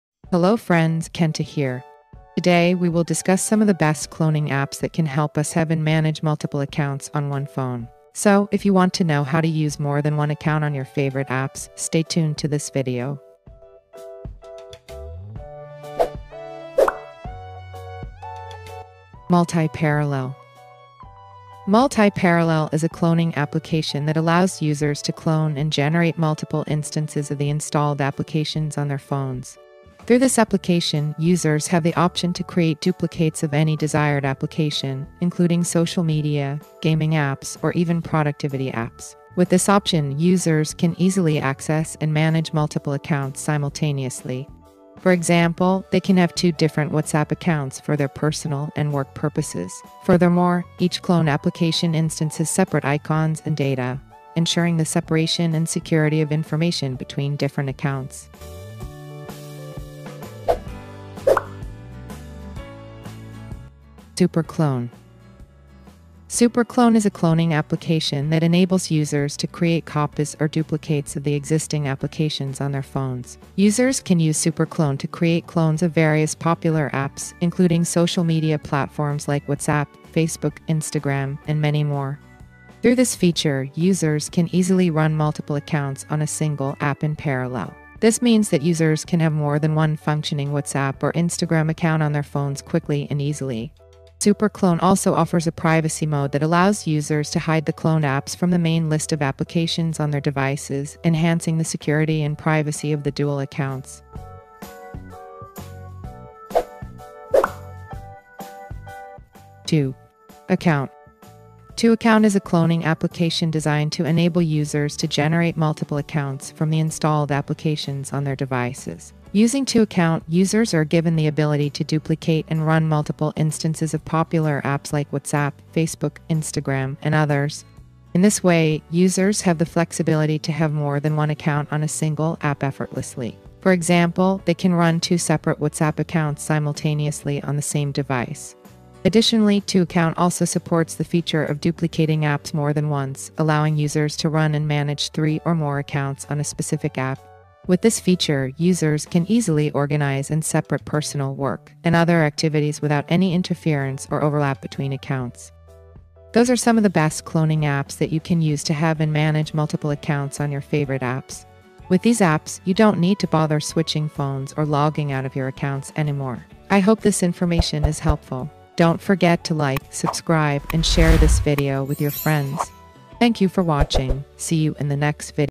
3 Best Clone App for Android in 2026 | Unlimited Clone App
This video discusses the top 3 best cloning applications for Android. These cloning applications allow users to duplicate applications and open two or more at the same time. Cloning applications have been widely used by people to duplicate their favorite applications.